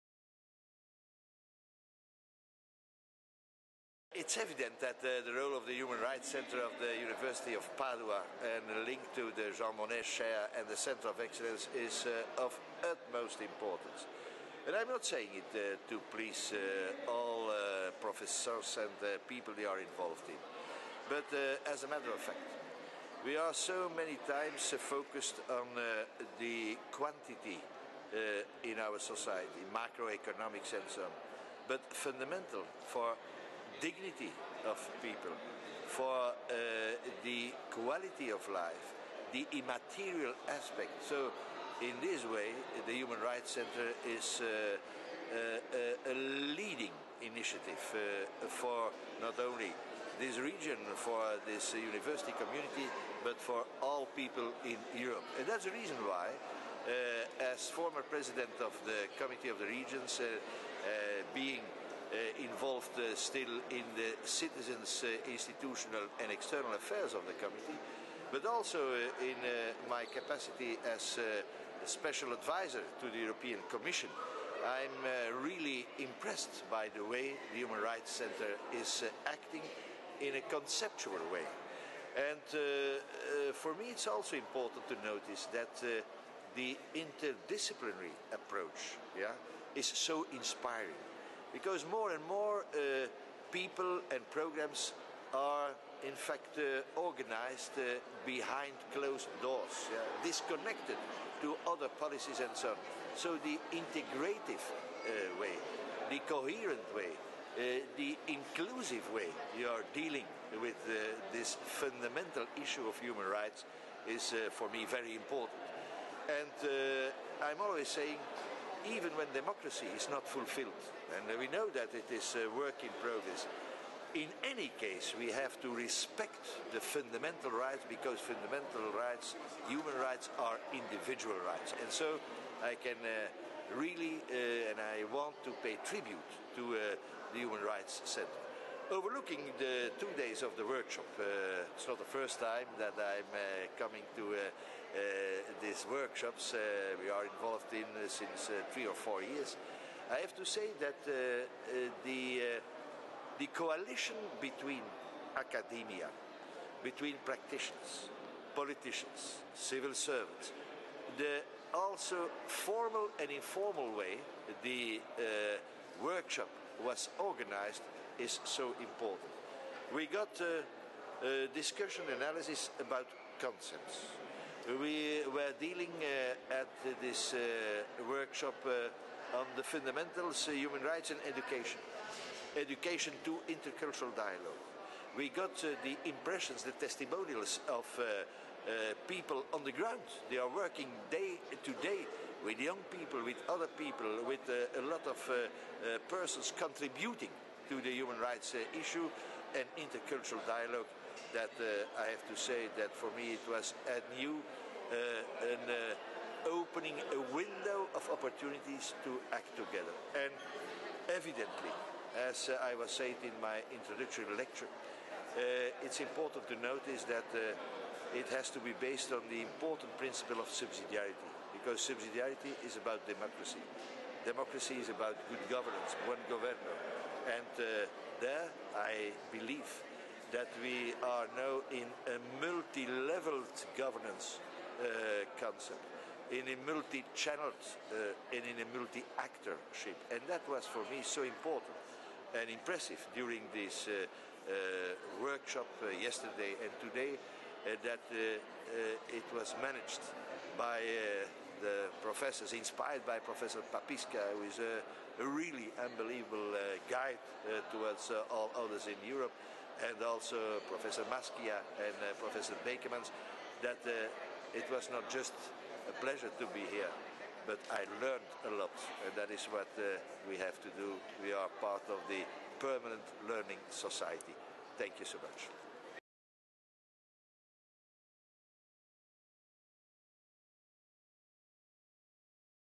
Luc Van den Brande: Dialogo interculturale nella cooperazione territoriale
Intervista a Luc Van den Brande durante il Workshop internazionale "Education to Intercultural Dialogue" (22-23/03/2011) che ha affrontato il tema degli spazi educativi e delle pratiche di apprendimento del dialogo interculturale in un mondo interdipendente. Il workshop è stato organizzato dal Centro Europeo di Eccellenza Jean Monnet e il Centro Diritti Umani dell'Università di Padova.